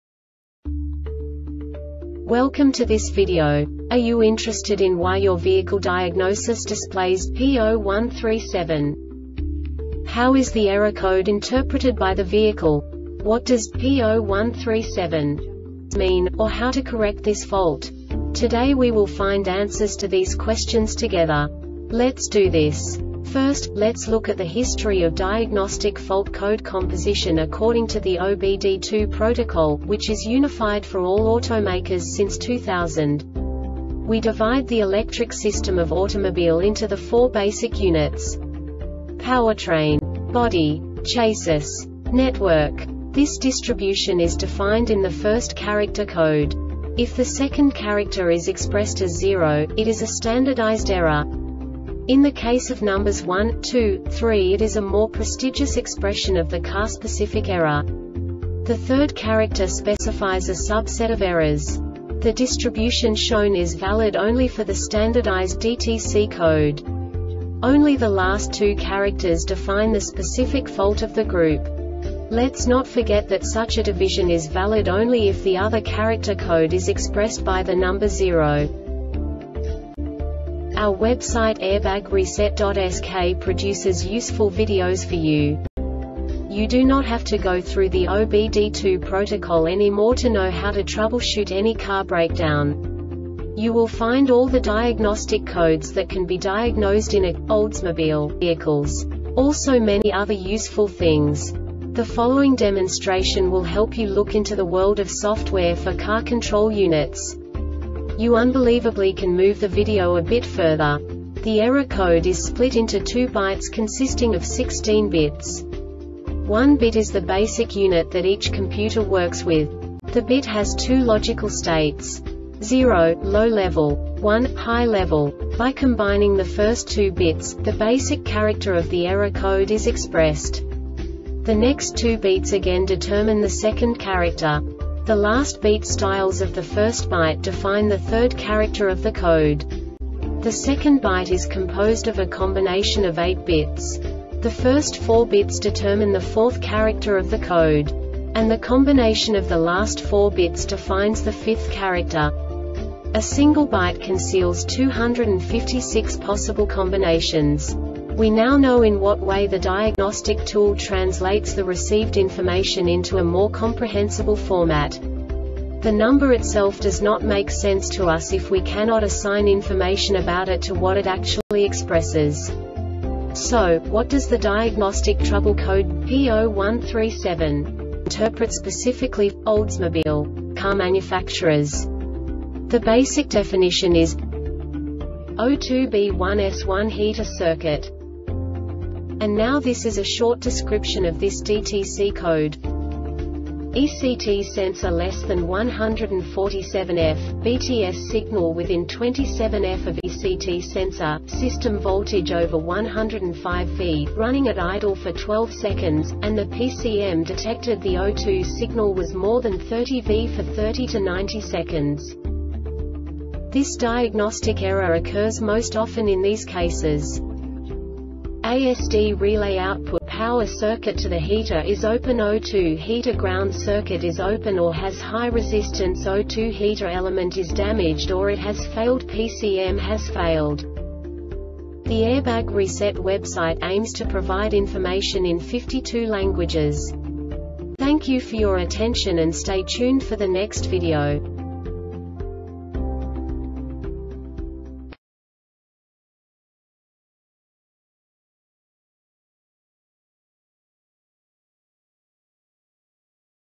DTC Oldsmobile P0137 Short Explanation
Contents:
0:21 Basic DTC analysis according to OBD2 protocol standard.
1:48 Insight into programming
2:58 A diagnostically understandable description of the Diagnostic Trouble Code
3:05 Basic error definition

Make:
Oldsmobile
Code:
P0137
Definition:
O2 (B1 S1) Heater Circuit
Description:
ECT Sensor less than 147ºF,BTS signal within 27ºF of ECT Sensor,system voltage over 10.5v,running at idle for 12 seconds,and the PCM detected the O2 signal was more than 3.0v for 30-90 seconds.
Cause:
ASD relay output (power) circuit to the heater is openO2 heater ground circuit is open or has high resistanceO2 heater element is damaged or it has failedPCM has failed